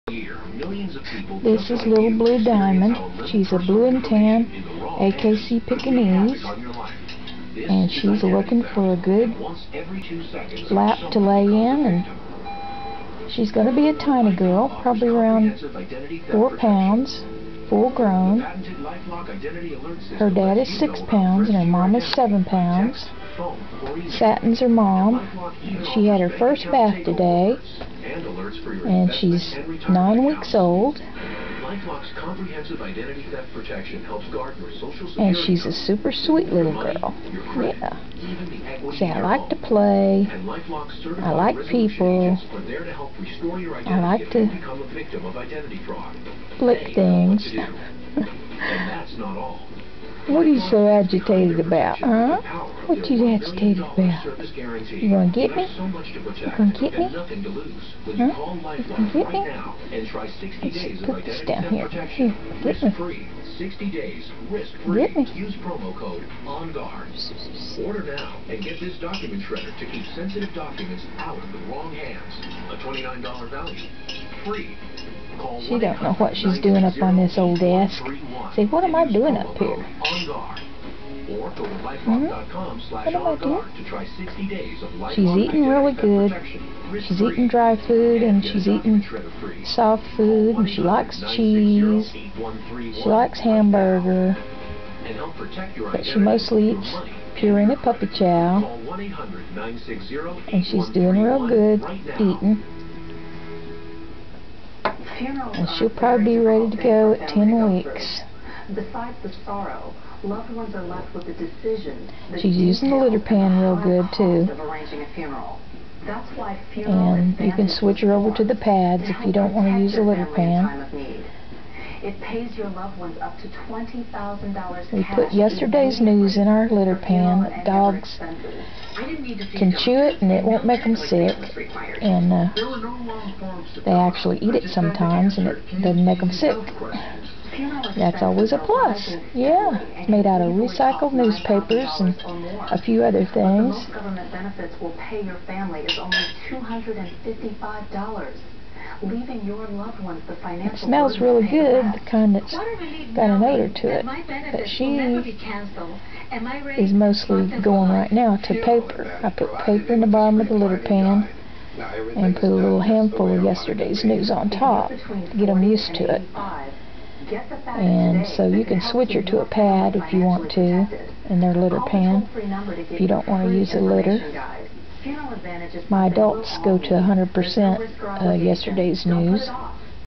LKLs October Gold Pekingese,AKC Satins Blue Diamond 9wk Apr 17
AKC Little Blue Diamond is a tiny sleeve pek. Litter Box Trained and has a Health Guarantee.